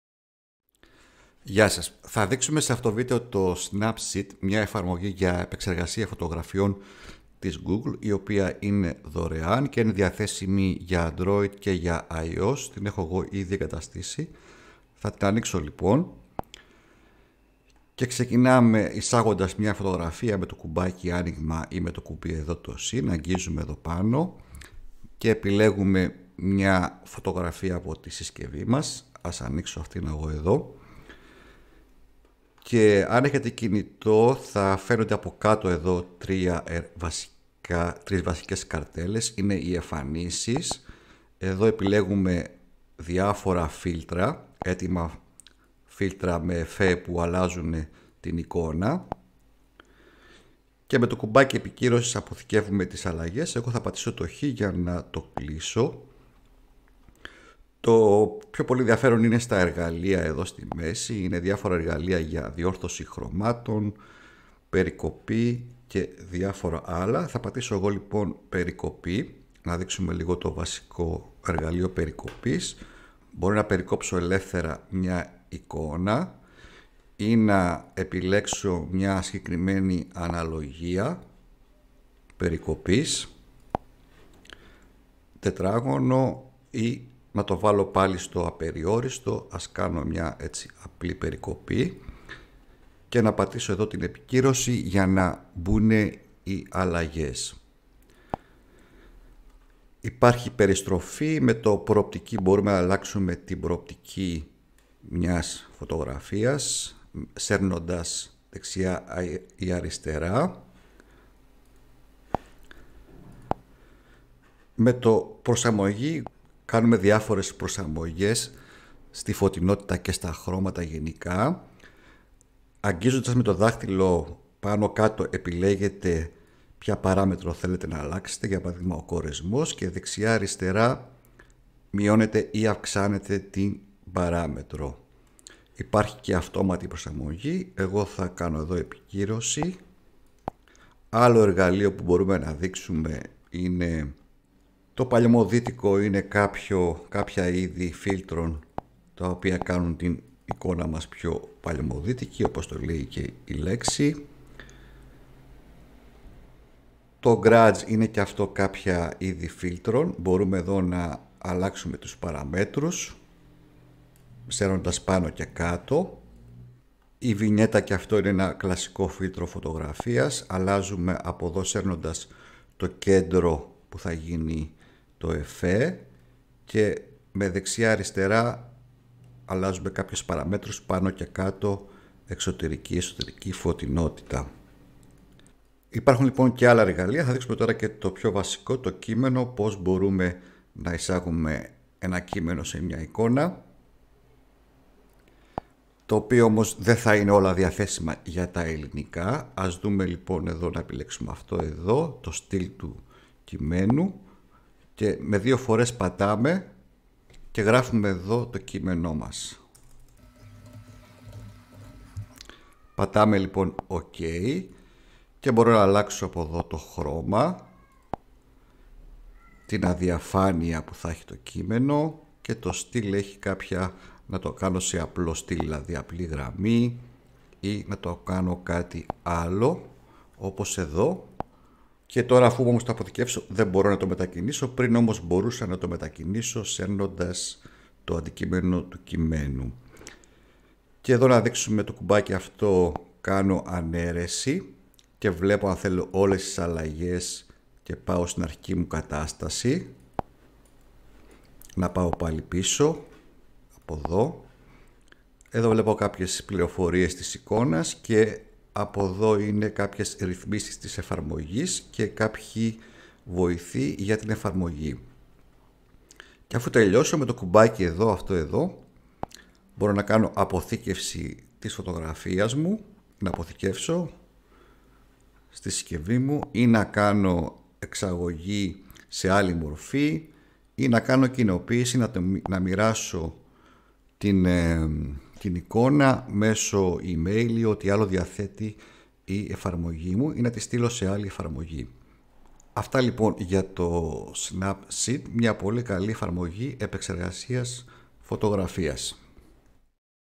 Επεξεργασία φωτογραφίας στο κινητό τάμπλετ με το Snapseed new
To Snapseed είναι μια δωρεάν εφαρμογή επεξεργασίας εικόνας για κινητά και τάμπλετ της Google. Είανι δωρεάν και διαθέσιμη για Android και iOS. Διορθώστε τον φωτισμό και τα χρώματα, εφαρμόστε εφέ, προσθέστε κείμενο κ.α.